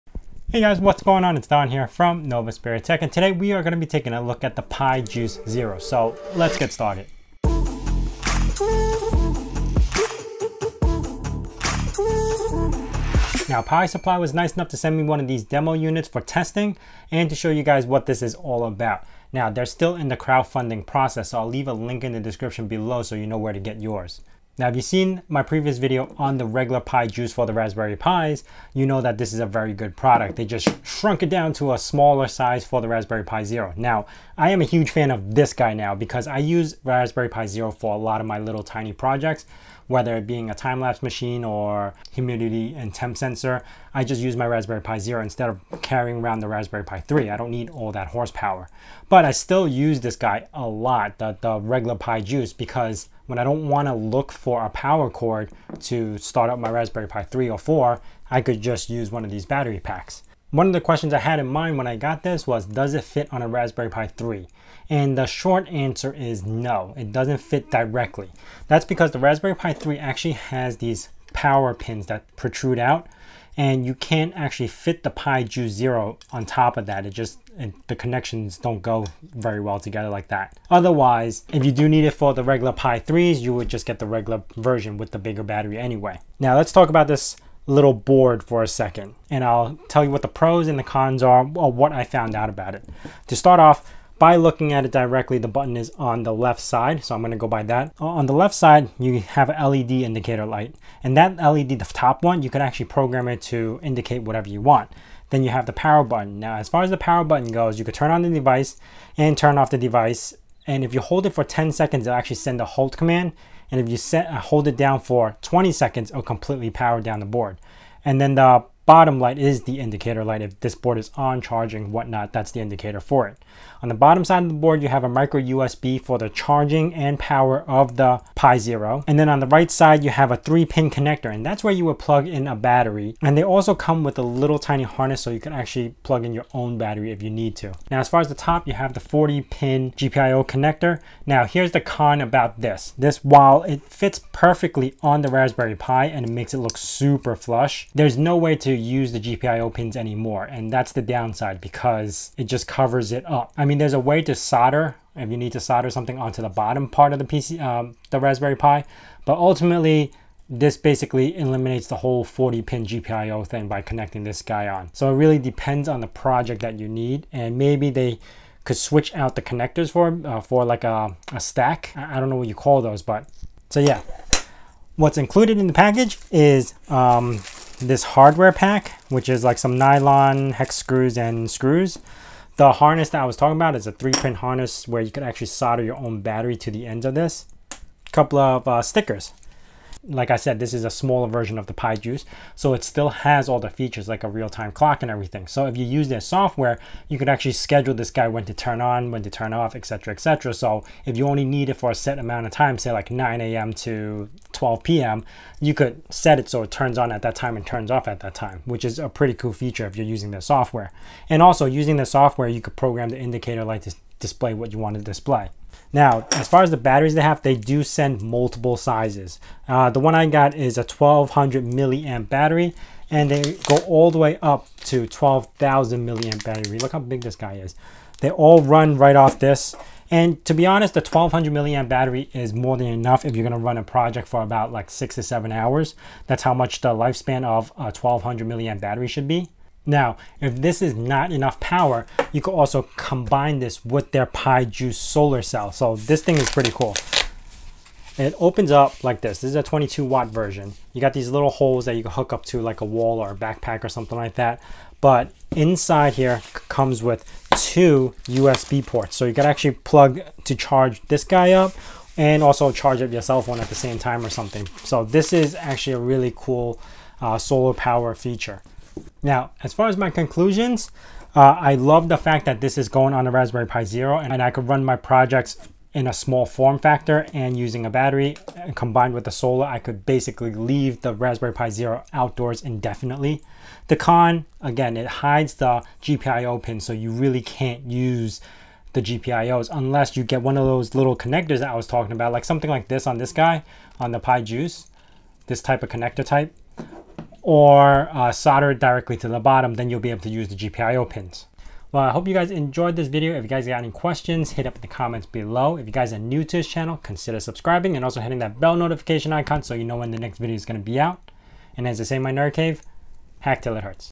Pi Juice Zero Hat
Pi Jucie zero hat first look!! this is something i have been waiting for!! a small compact hat for raspberry pi zero!!

○○○ SHOP ○○○